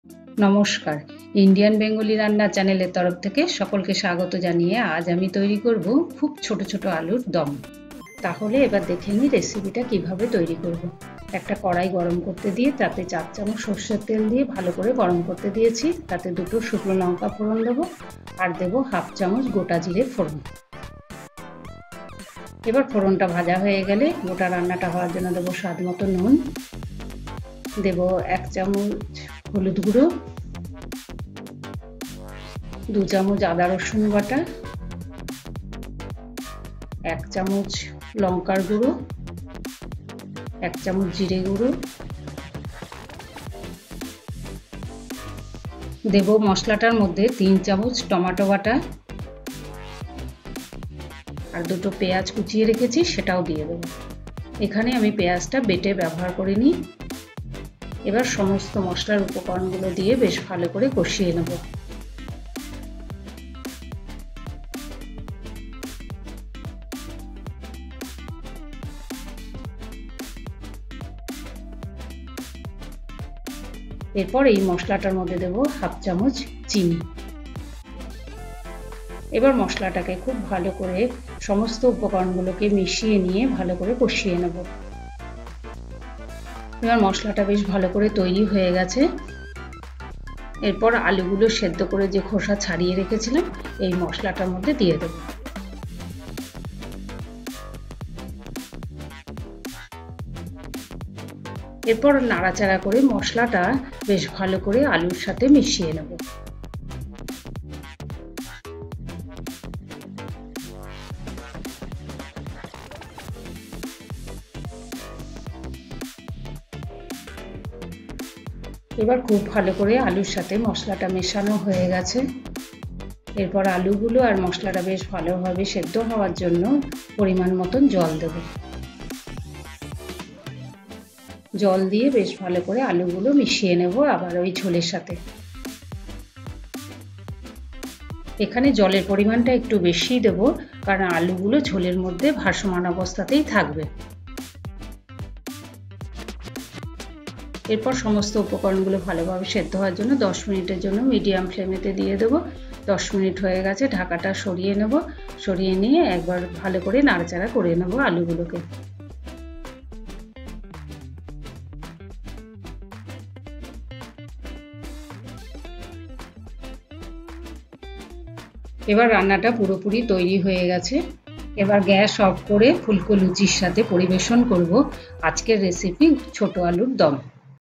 সকালবেলায় অল্প সময়ে টিফিনের জন্য চটজলদি আলুর দমের রেসিপি
ছোট ছোট আলুর চটপটি দম | গুড়ি আলুর দম | Choto choto Alur Dum

INDIAN BENGALI RANNA